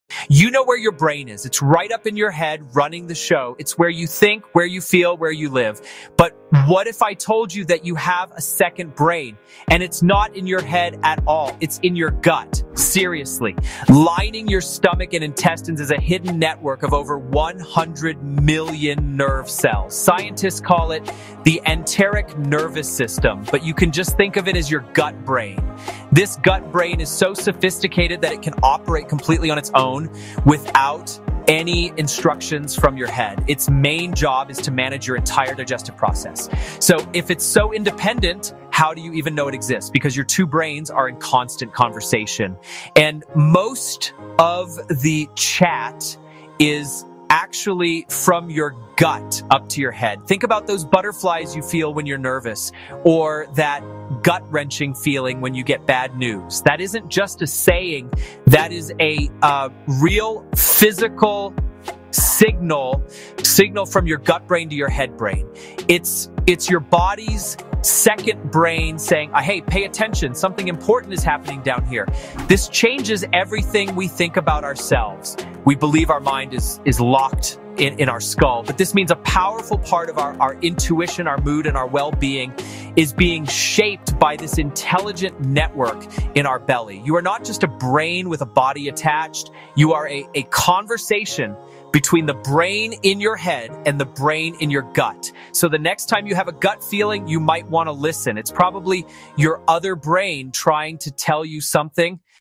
Did You Know Your Gut Has a Brain Too?
Discover the surprising truth: you have a second brain — in your gut. This 1:50 short dives into the Enteric Nervous System, the “gut brain” of 100+ million nerve cells that runs digestion and talks non-stop to your head. Learn why butterflies, gut-wrenching shock, and that deep intuition are real signals from your belly, and how the gut-brain axis shapes mood, decision-making, and wellbeing. Perfect for curious science fans and anyone fascinated by neuroscience, intuition, and the microbiome.
See Less

Discover the fascinating concept of your 'second brain' located in your gut, a complex network of nerve cells crucial for your overall well-being and feelings. This video explains the **gut brain axis** and its independent operation, highlighting its role in **digestion** and other functions. Understanding this intricate connection is a significant step in comprehending your body's **physiology** and improving your **gut health**.